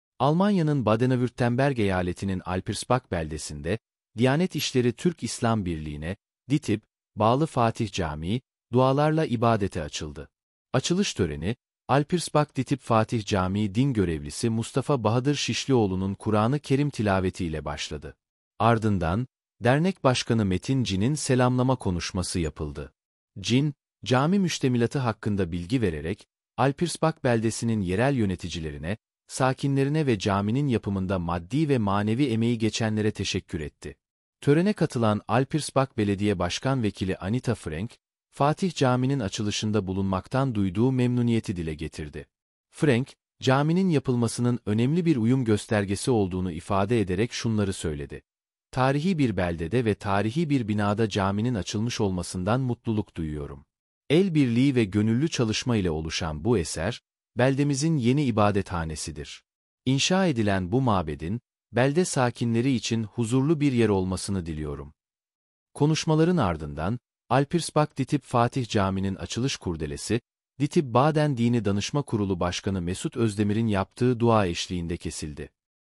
Alpirsbach DİTİB Fatih Camii, dualarla ibadete açıldı
Almanya'nın Baden-Württemberg eyaletinin Alpirsbach beldesinde Diyanet İşleri Türk İslam Birliği‘ne (DİTİB) bağlı Fatih Camii, dualarla ibadete açıldı.

Açılış törenine; Türkiye Cumhuriyeti Karlsruhe Başkonsolosu Mahmut Niyazi Sezgin, DİTİB Genel Başkanı Dr. Muharrem Kuzey, Freudenstadt Belediye Başkan Yardımcısı Gerhard Gaiser, Alpirsbach Belediye Başkanı Michael Emil Pfaff, Belediye Başkan Vekili Anita Frank, Alpirsbach eski Belediye Başkanı Ulrich Reiner, DİTİB Baden Dini Danışma Kurulu Başkanı Mesut Özdemir, Baden Eyalet Bölge Birliği Başkanı Nihat Demirci, DİTİB Baden-Württemberg İslam Dini Cemaati Eyalet Müdürü Fatih Şahan, Katolik Kilisesi’nden Dekan Anton Bock, Protestan Kilisesi’nden Dr. Alex Kohler ve Alpirsbach Fatih Camii Dernek Başkanı Metin Cin’in yanı sıra yerel yönetimden temsilciler, çevre DİTİB derneklerinin başkan ve yöneticileri ile Alpirsbach‘da yaşayan Alman ve Türk sakinler katıldı.

Açılış töreni, Alpirsbach DİTİB Fatih Camii din görevlisi Mustafa Bahadır Şişlioğlu’nun Kur’an-ı Kerim tilavetinin ardından dernek başkanı Metin Cin’in selamlama konuşması ile başladı. Cami müştemilatı hakkında bilgi veren Cin, Alpirsbach beldesinin yerel yöneticilerine, sakinlerine ve caminin yapımında maddi ve manevi emeği geçenlere teşekkür etti.

Alpirsbach, tarihi, kültürel ve doğal güzellikleriyle ziyaretçilerine zengin deneyimler sunan yaklaşık 7 bin nüfuslu beldede, daha önce hastane olarak inşa edilmiş ve uzun yıllar hizmet vermiş binayı 2013 yılında satın alıp camiye dönüştürdüklerini söyleyen Cin, “1000 metrekare alan üzerine, 600 metrekare kullanım alanı ve 400 metrekare ferah bahçesi ile üç kattan oluşan camide, mescid, derslikler, gençlere ve yetişkinlere özel lokal, idari büro gibi hizmet alanlarının bulunduğunu söyledi. Başkan Cin, tadilat, tezyinat ve yenileme çalışmalarını bitirerek caminin açılışını gerçekleştirdiklerini sözlerine ekledi.

Törene katılan Alpirsbach Belediye Başkan Vekili Anita Frank, Fatih Camii’nin açılışında bulunmaktan duyduğu memnuniyeti dile getirdi. Caminin yapılmasının önemli bir uyum göstergesi olduğunu ifade eden Frank, “Tarihi bir beldede ve tarihi bir binada caminin açılmış olmasından mutluluk duyuyorum. El birliği ve gönüllü çalışma ile oluşan bu eser beldemizin yeni ibadethanesidir. İnşa edilen bu mabedin, belde sakinleri için huzurlu bir yer olmasını diliyorum” ifadelerini kullandı.

Açılış törenine katılan davetliler de yaptıkları kısa konuşmalarda tarihi Alpirsbach beldesine caminin ayrı bir zenginlik katacağını ifade ederek, caminin yapımında emeği geçen cami yönetimini ve cemaatini kutlayarak, hayırlı olsun dileğinde bulundular.

Türkiye Cumhuriyeti Karlsruhe Başkonsolosu Mahmut Niyazi Sezgin, Alpirsbach Camii'nin resmi açılışında yaptığı konuşmada, caminin sadece Müslümanlar için değil, tüm Alpirsbach şehri için bir buluşma ve kültür merkezi olacağını vurguladı. Sezgin, Baden'de 10 aydır görev yaptığını, beldede Türk yurttaşların başarı öykülerinden büyük bir ilham aldığını ve Alpirsbach’ı üçüncü kez ziyaret ettiğini belirtti. Camilerin barış, bir arada yaşama ve karşılıklı saygının simgesi olduğuna vurgu yapan Başkonsolos Sezgin, “Bu güzel caminin beldenin bir buluşma yeri ve kültür merkezi olarak büyük fayda sağlayacağını ve birlikte yaşama kültürüne katkıda bulunmaya devam edeceğini umuyorum” dedi.

Türk halkını barışçıl, dost canlısı ve çalışkan olarak tanımlayan Sezgin, Alpirsbach’ta başkan Metin Cin’i, Türk ve Alman toplumu için yaptığı gönüllü çalışmalardan, dürüstlüğü ve samimiyetinden etkilendiğini, daha iyi birlikte yaşam için hayatı boyunca yorulmadan katkıda bulunduğunu dile getirdi.

Gönüllü çalışmalarından dolayı tüm hayırseverlere teşekkür eden Sezgin, “Caminin inşasında emeği geçen herkesi tebrik ediyorum. Hayırlı ve uğurlu olsun” ifadelerini kullandı.

DİTİB'in 40'ıncı kuruluş yılını aynı zamanda komşuluk geleneğinin de 40 yılı olarak gördüklerini söyleyen Kuzey, “Almanya'da 860'dan fazla camisiyle en büyük Müslüman dini topluluk olan DİTİB, burada yaşayan Müslümanların ve tüm toplumun refahını her zaman göz önünde bulunduruyor. DİTİB, bireylere, dini cemaatlere ve topluma hizmet eden dini ve sosyal hizmetler sunmaktadır. Bu yıl 40. yılını kutlayan DİTİB, geçmişine gururla bakarak geleceği planlıyor. Bu camimiz, bereket, umut ve dayanışma yeri olsun. Günlük yaşamımızda en yüksek ahlaki prensiplere ulaşma çabamızda bize ilham versin” diye konuştu.

Jordingstr. 5
47119 Duisburg

Frekans: 12729
Sembol: 30000
Polarizasyon: H
FEC: 2/3